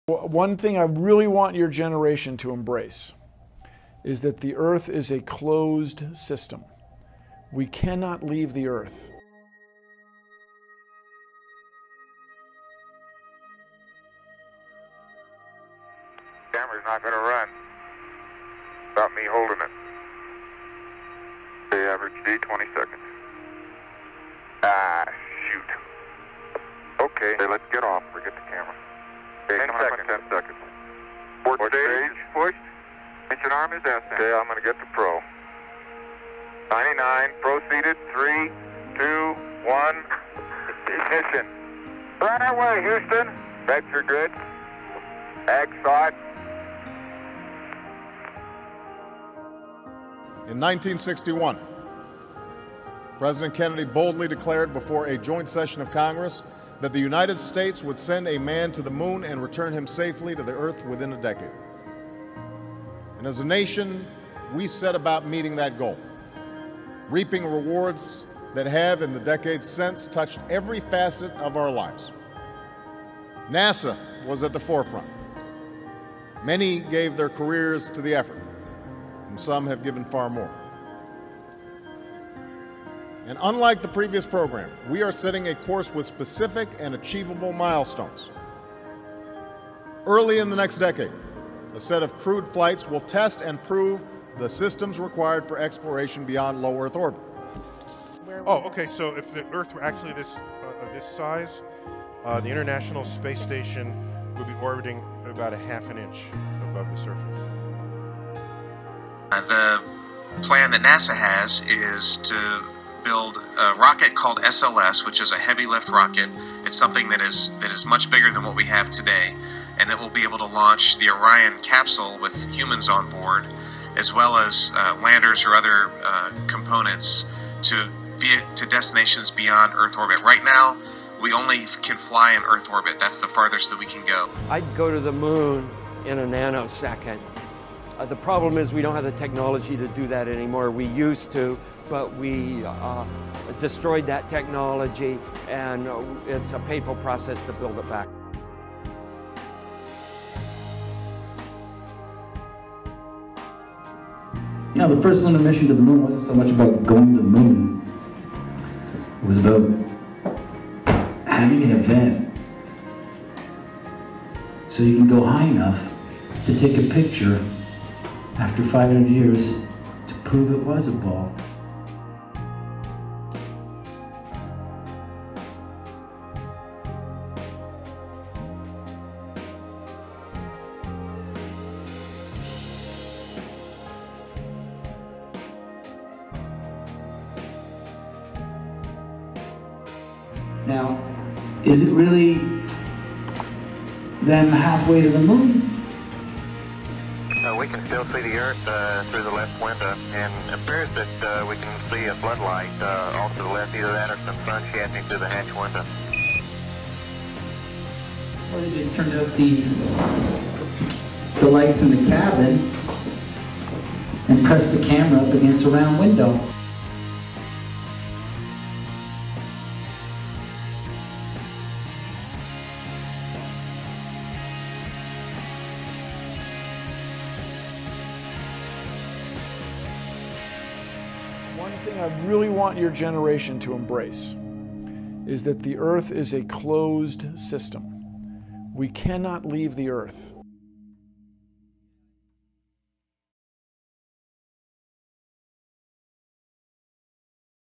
Tears of the Moon
If we went to the moon 50 years ago, why have we not been back and why do we not have the technology to go back?